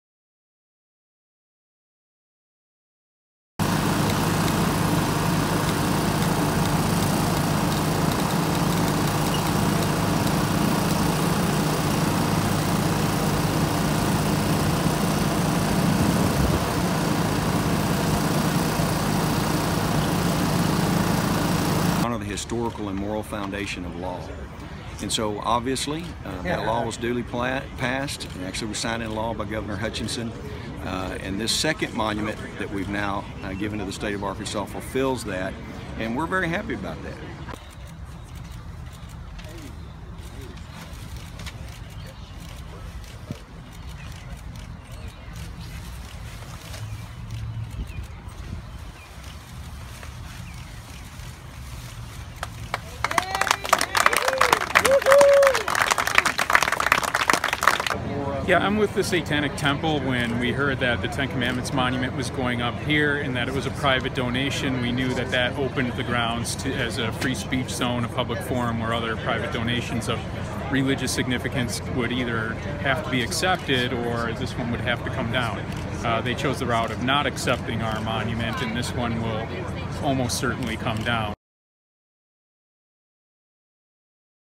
Ten Commandments monument installed on Arkansas Capitol grounds
For the second time in less than a year, a monument to the Ten Commandments has been unveiled on the grounds of the Arkansas state Capitol.